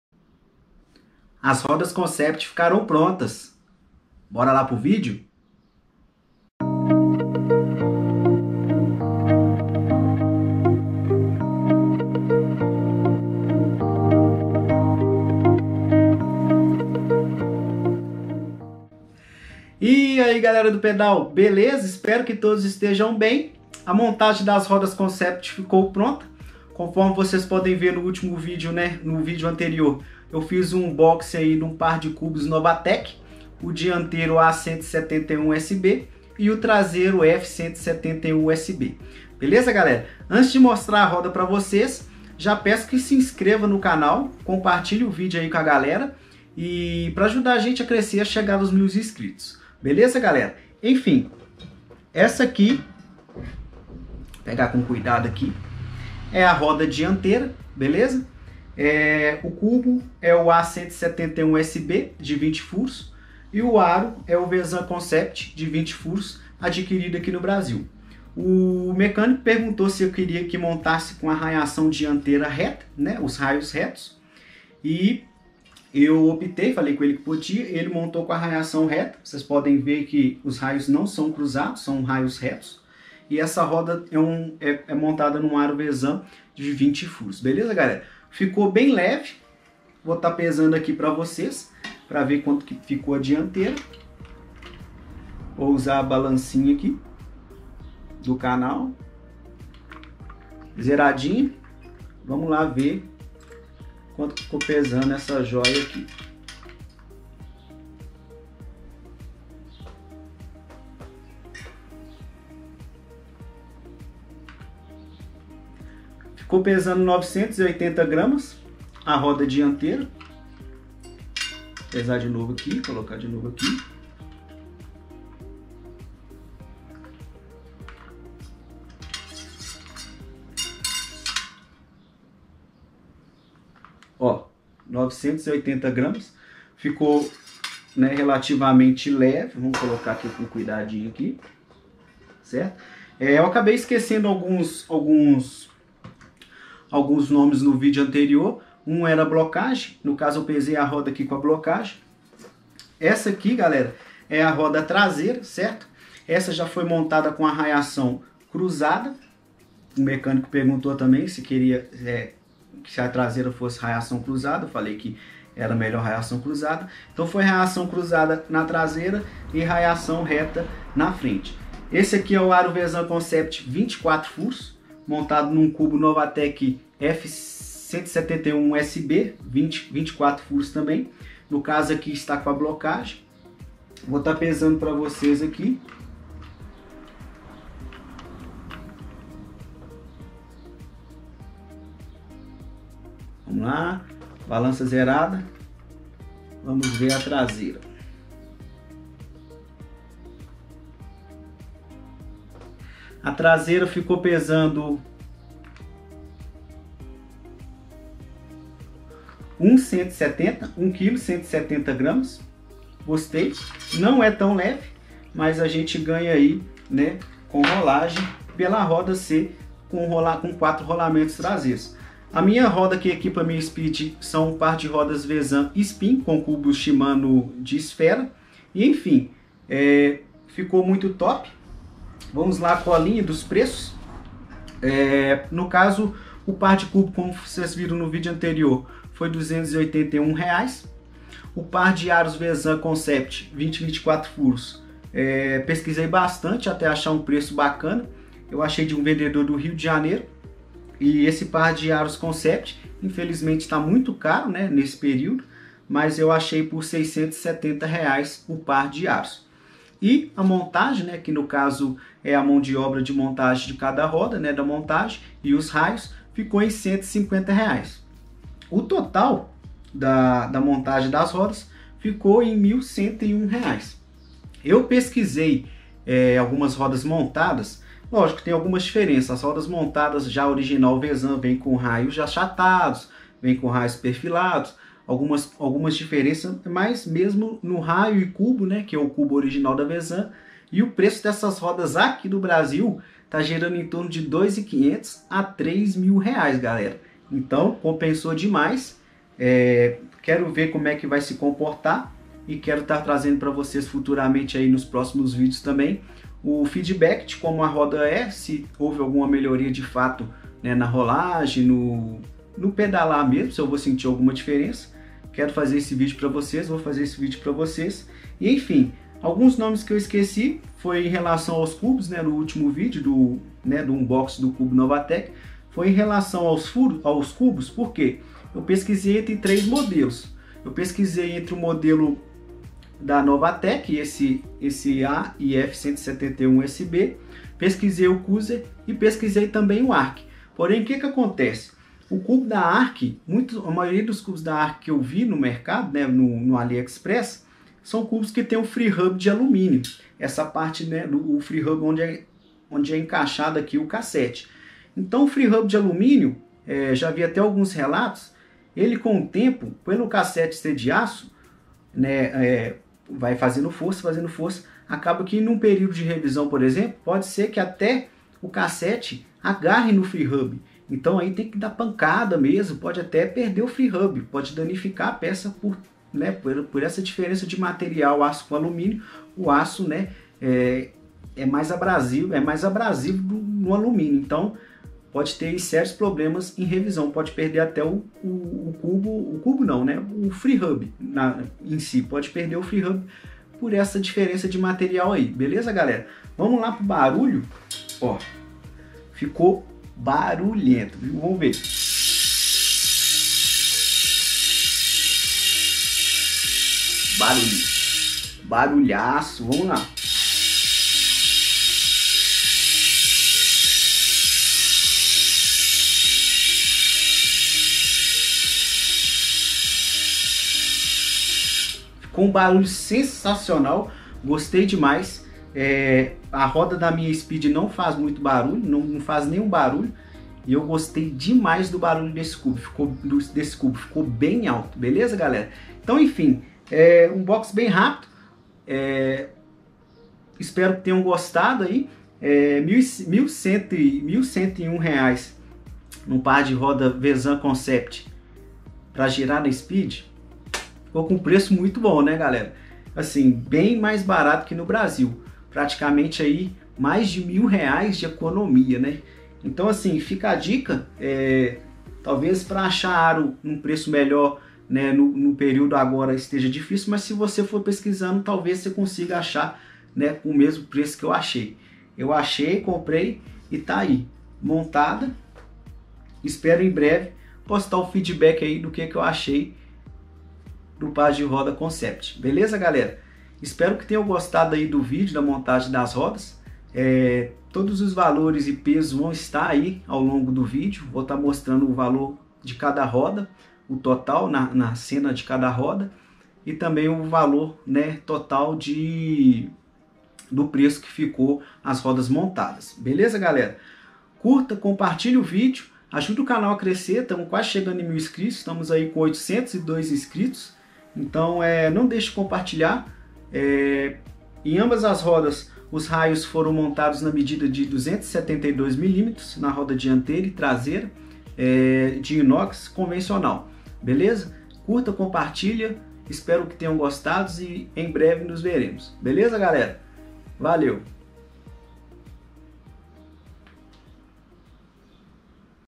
Roda Vzan Concept + Cubos Novatec a171sb/f172sb 20/24 Furos com Raios Inox 272mm #058
Vídeo da montagem final das rodas vzan concept, montadas com cubos Novatec a171sb/f172sb 20/24 furos importados do aliexpress.

Valor final da montagem:

Cubo: R$ 281,00
Aros:  R$ 670,00
Montagem + Raios: R$ 150,00

Total: R$ 1.101,00

Peso final com blocagem:

Dianteira: 980g
Traseira: 1.170g

Total: 2.150g

Se comparado as rodas prontas no Brasil, o preço saiu bem mais em conta. Compensa muito montar as rodas importando as peças.

Espero que tenha ajudado! TMJ!

Bons treinos!

Obrigado!